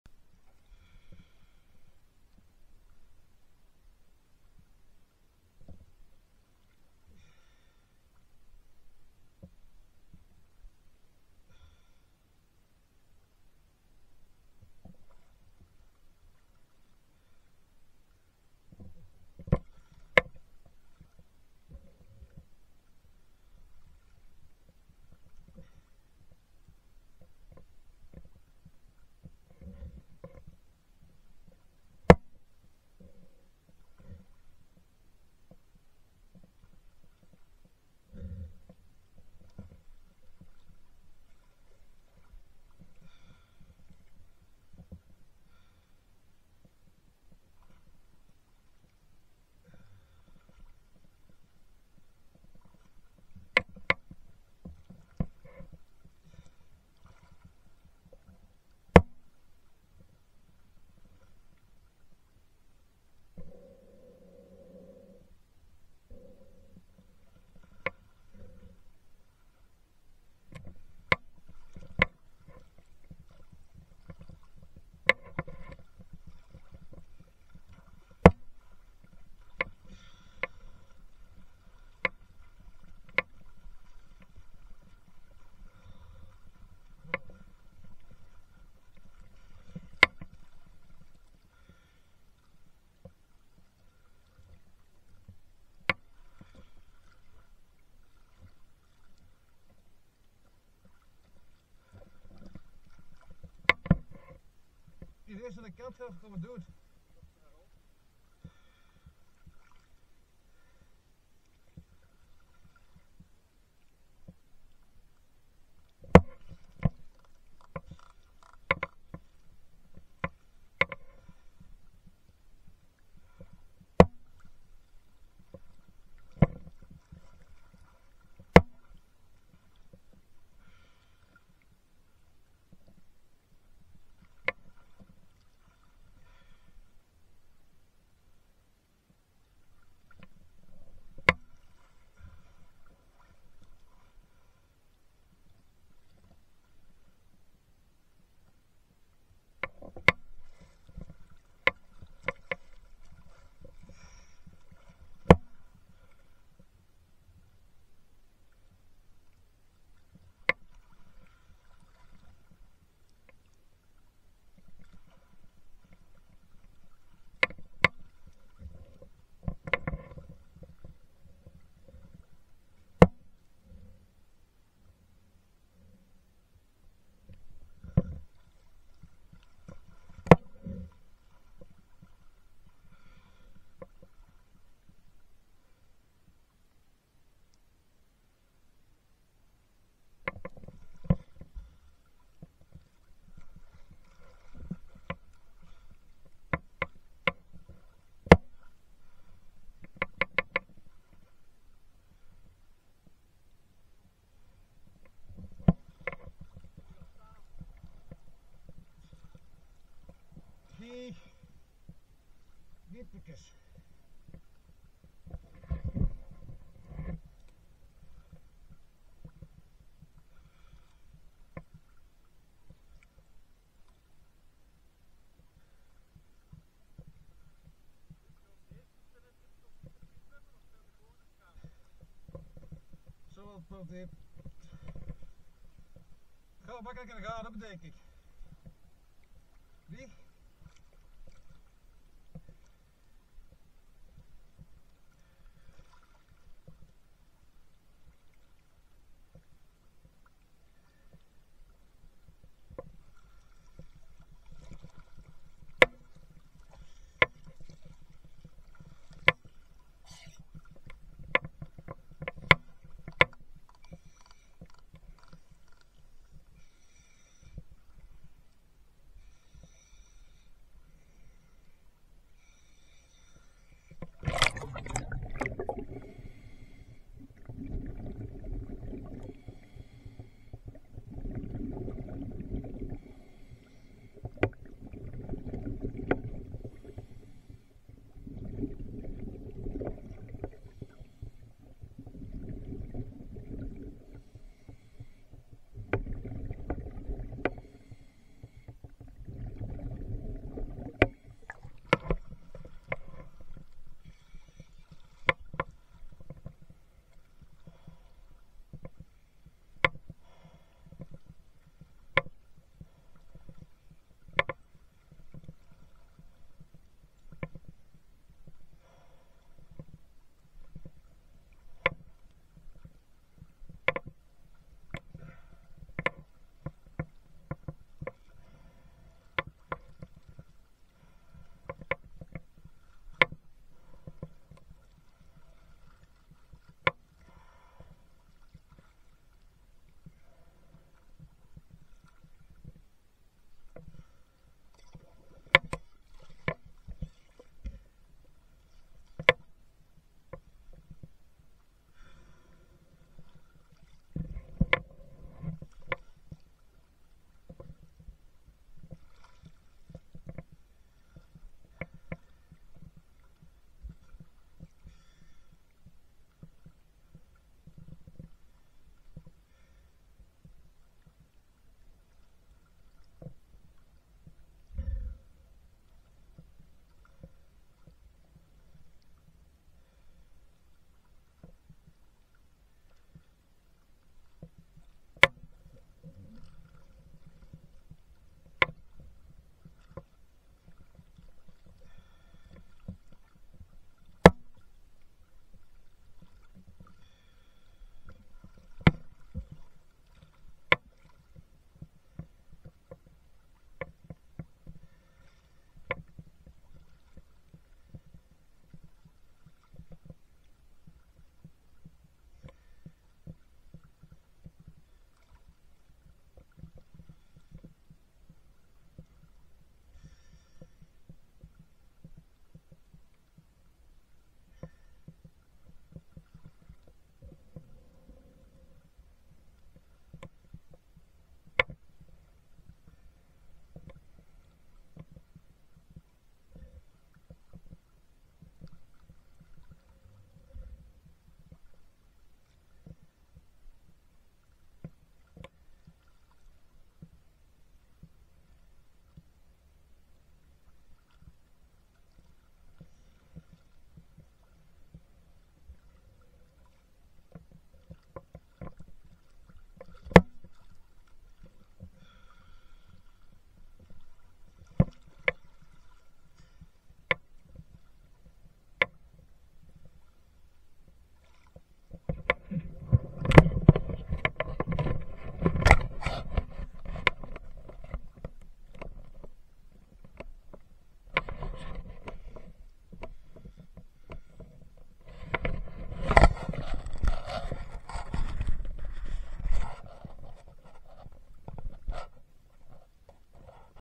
25 februari 2024 Netherlands Oesterdam headcam part 4
Diving in the netherlands, sadly no fish found